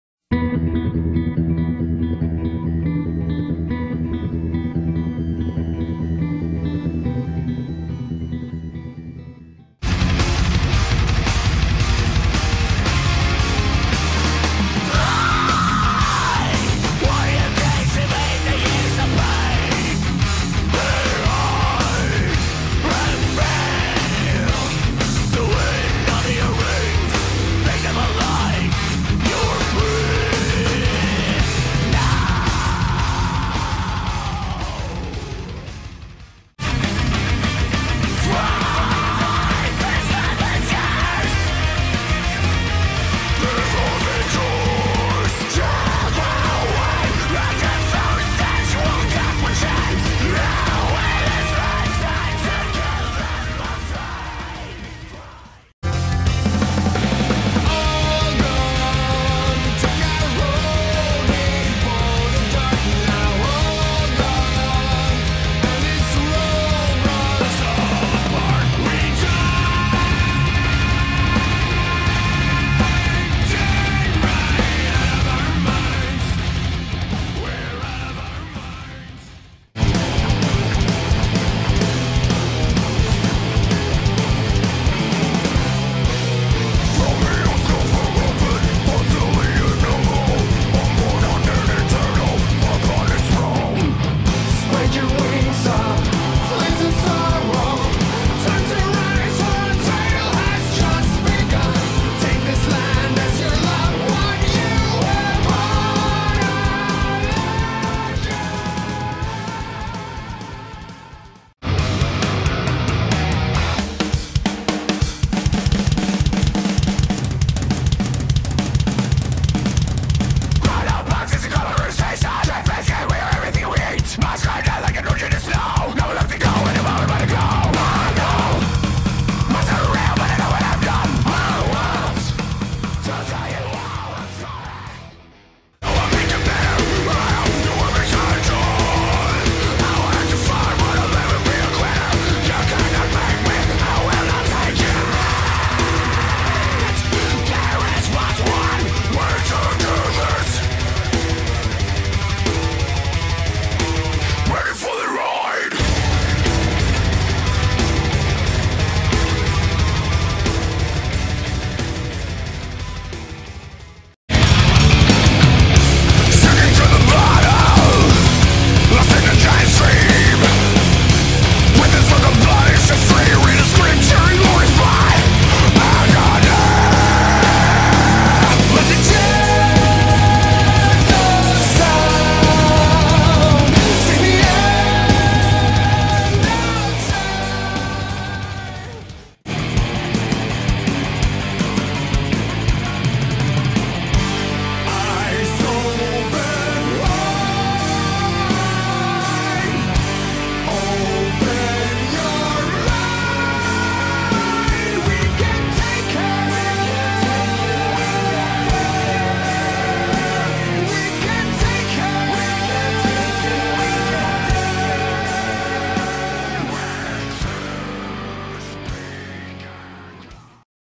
Avatar Albums Ranked
My ranking of Avatars albums from worst to best. How would you rank them?